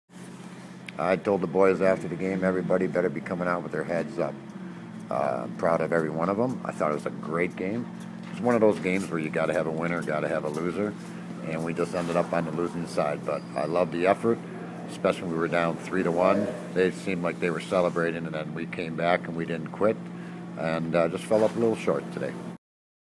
Team USA Head Coach Dave Debol Comments After Loss to Belarus at Winter World University Games
Team USA Head Coach Dave Debol shares his thoughts after the U.S. Men's National University Team falls to Belarus, 6-3, in the quarterfinals of the Winter World University Games.